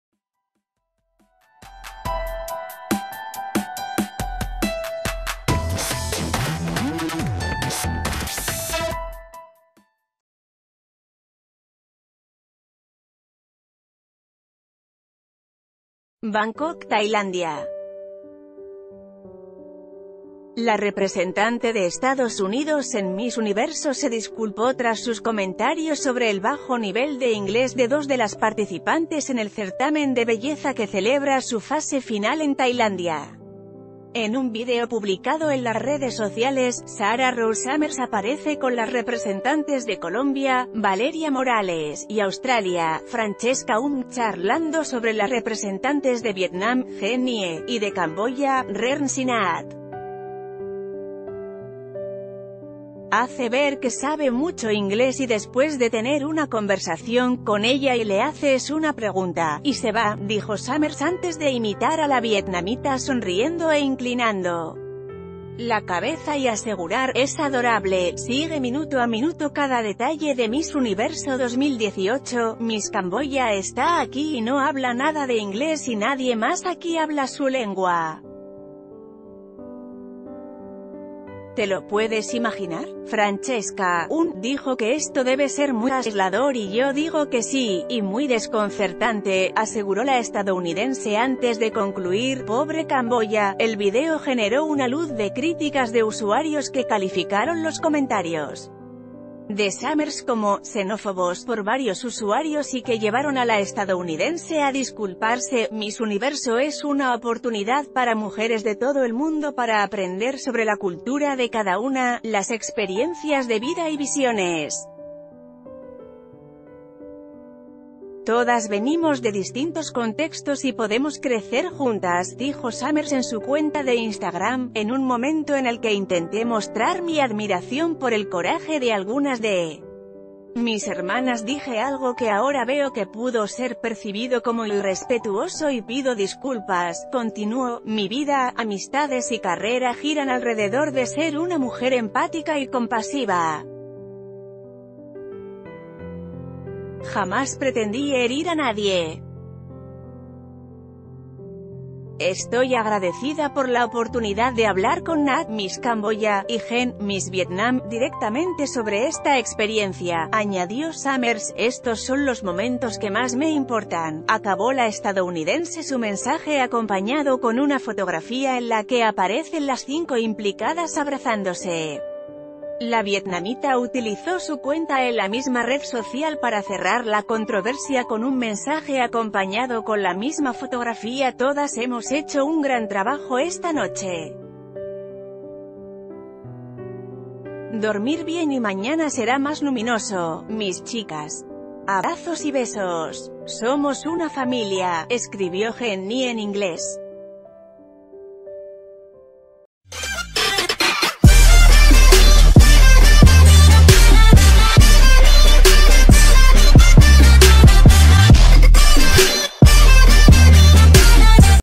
"Pobre Camboya", Miss EU se burla de sus compañeras en Miss Universo por no hablar inglés
Bangkok, Tailandia. La representante de Estados Unidos en Miss Universo se disculpó tras sus comentarios sobre el bajo nivel de inglés de dos de las participantes en el certamen de belleza que celebra su fase final en Tailandia.